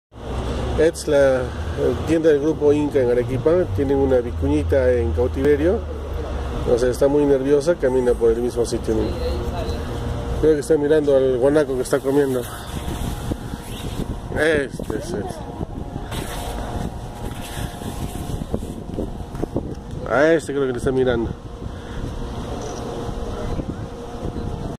Arequipa ciudad blanca
vicuña en la tienda del grupo inka en la que fabrican prendas de fibra de alpaca, etc.